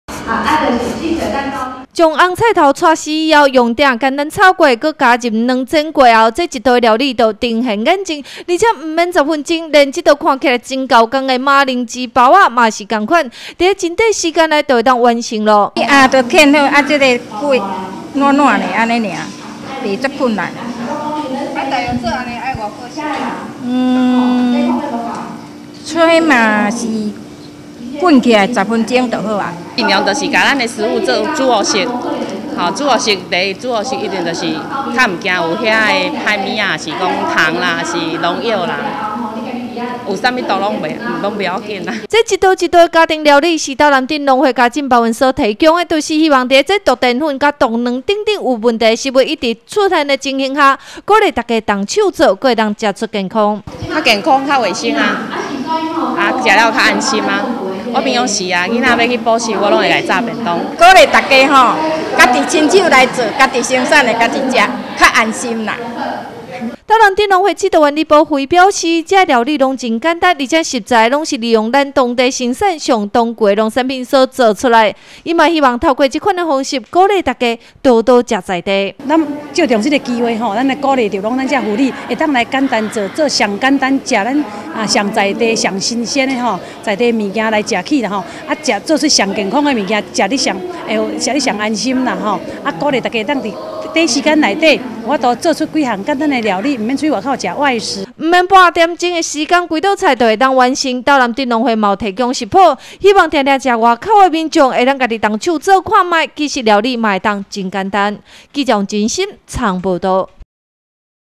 雲林新聞網-斗南吃在地最健康
毒澱粉、毒蛋等食品負面消息不斷傳出，也讓許多人不知道還有什麼能吃的，斗南鎮農會家政班員，就特別整理食譜，希望提供大家用最輕鬆的方式來做料理，而且不用三十分鐘的時間就能完成整桌菜餚，斗南鎮農會指導員李寶慧表示，雲林縣有許多優質的農特產，民眾應該要多多利用，因為吃在地當季生產的東西，最健康。【以上新聞可從哈TV App收視】